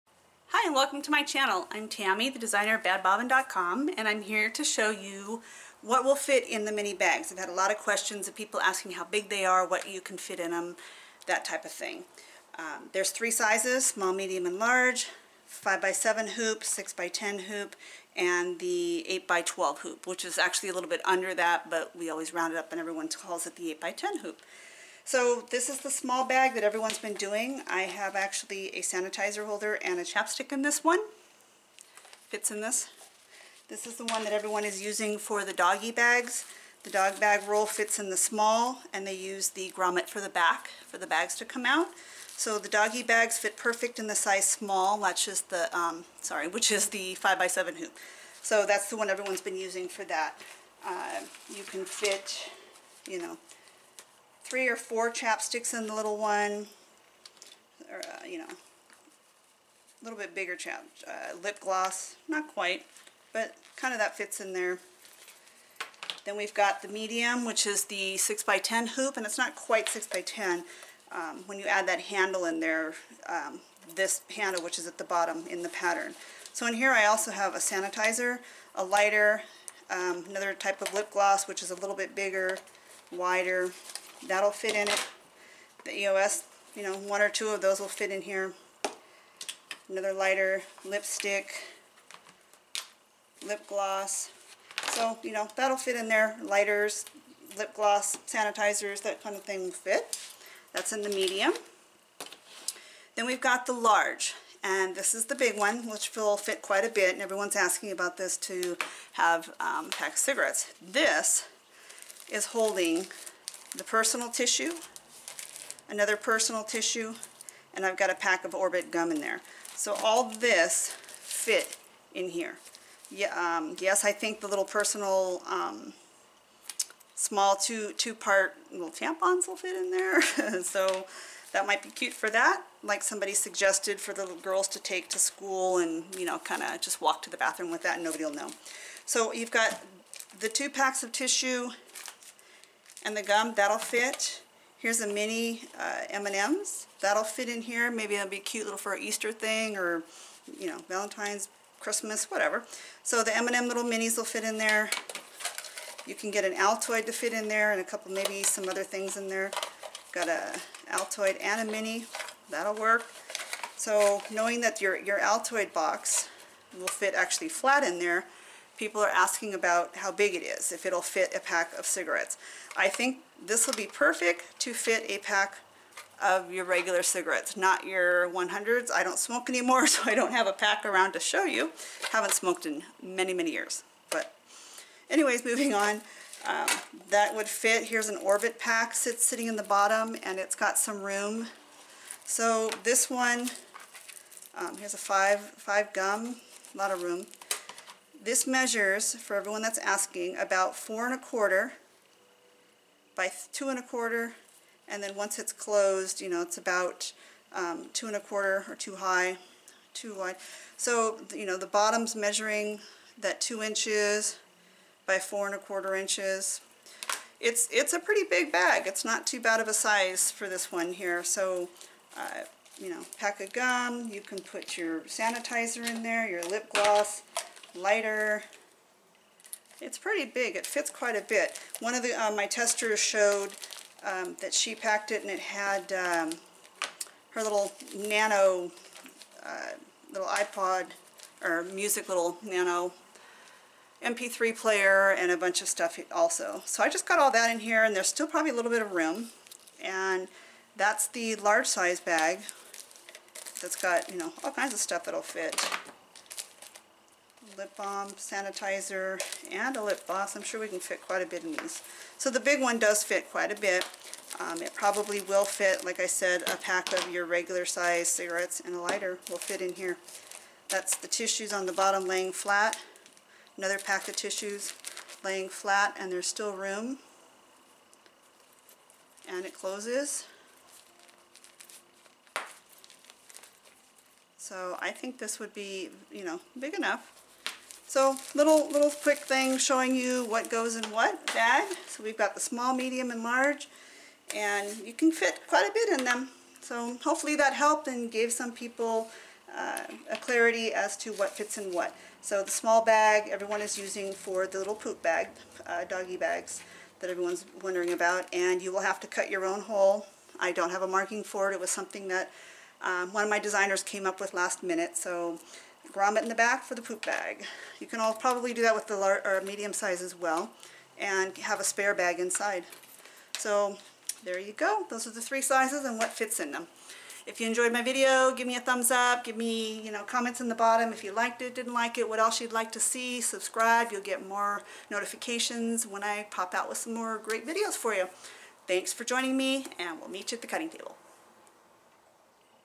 How Much Will Fit in the Mini Purse By Bad Bobbin
In this video I will be showing you how much will fit in all 3 sizes of the Mini Purse design by Bad Bobbin.